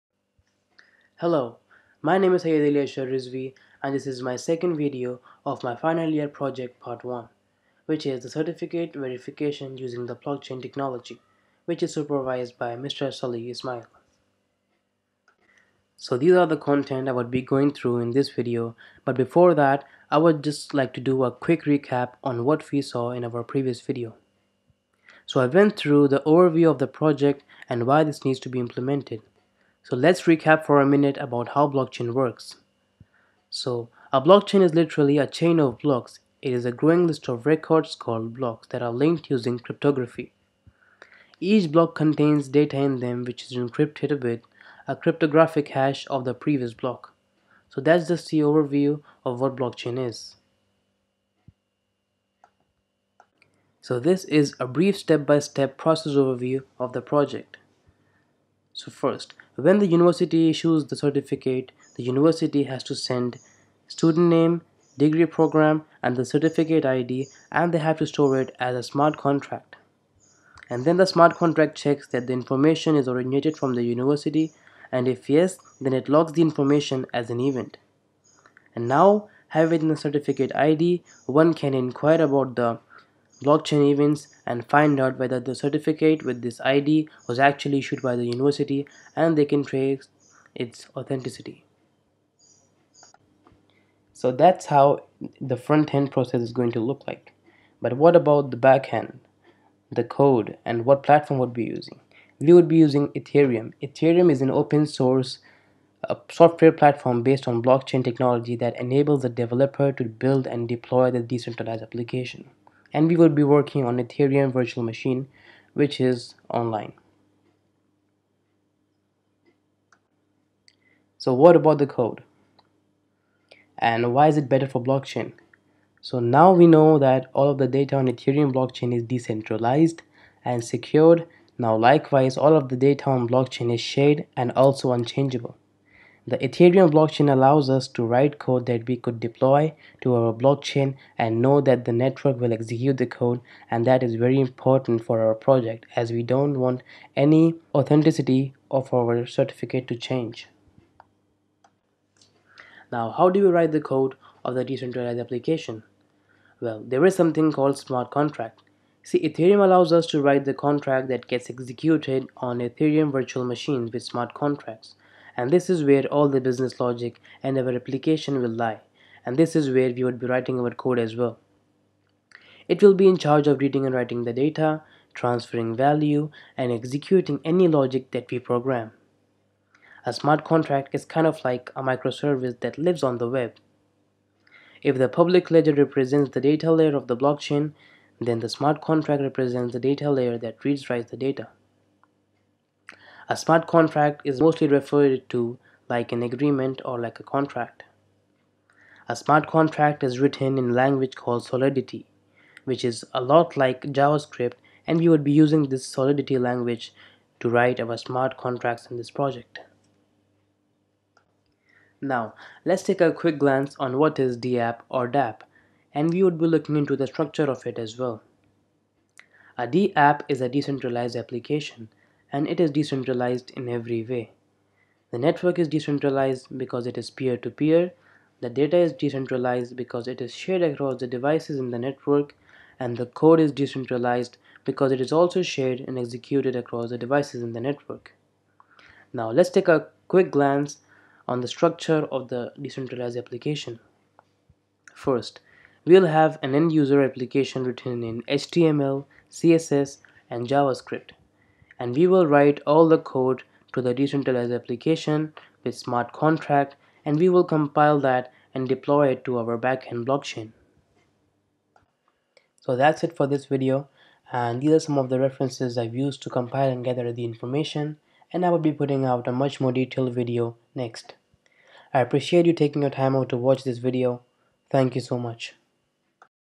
Certificate Verification using Blockchain - CTP 1 (Part 2)
Computer Technology Project (1)
Part 2

Syed Ali Ashar Rizvi, 19661548
Bachelors of Science in Computer Systems and Networking
Curtin University, Dubai